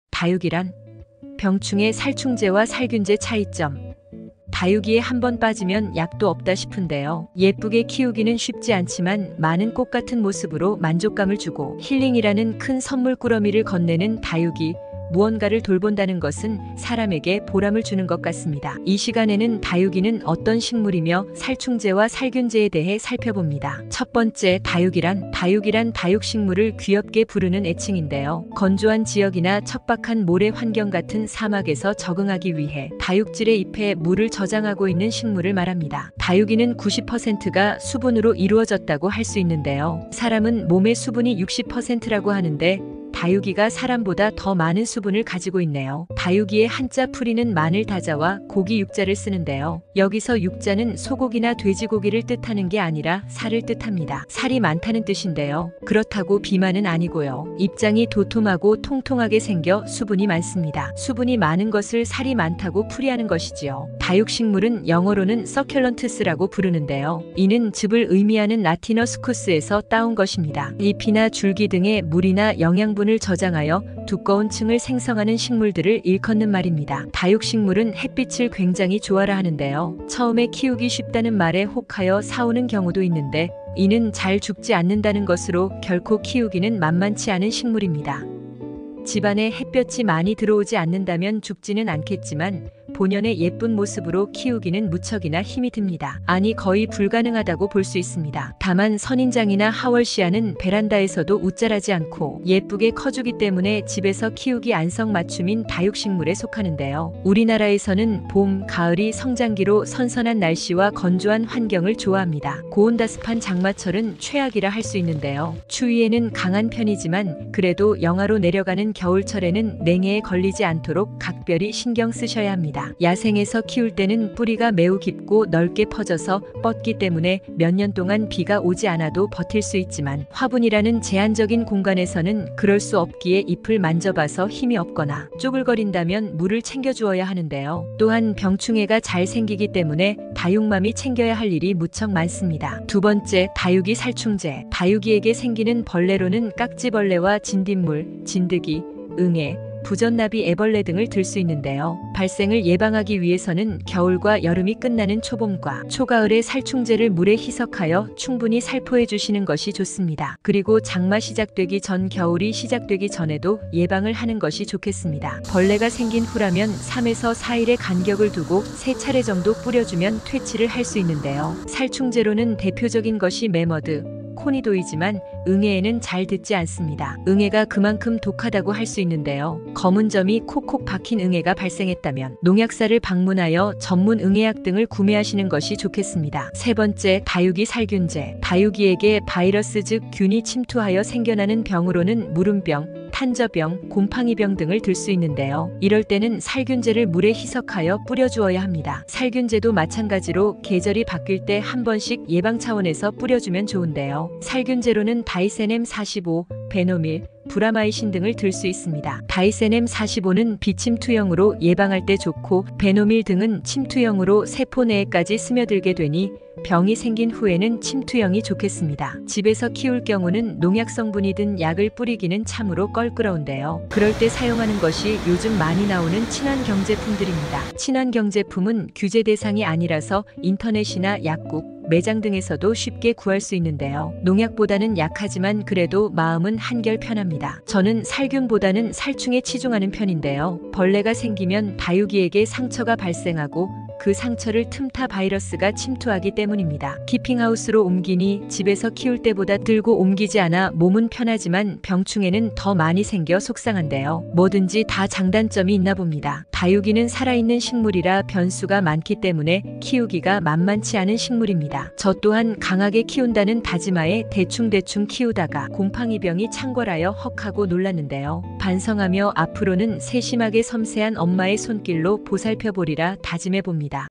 다육이의 의미!! 병충해와 살충제, 살균제 알아보기🪴
다육이에 반해서 시작하였다가 그 규모가 많이 커졌는데요. 다육이의 의미와 병충해, 병충해를 대처하는 살충제와 살균제에 대해 들여다봅니다.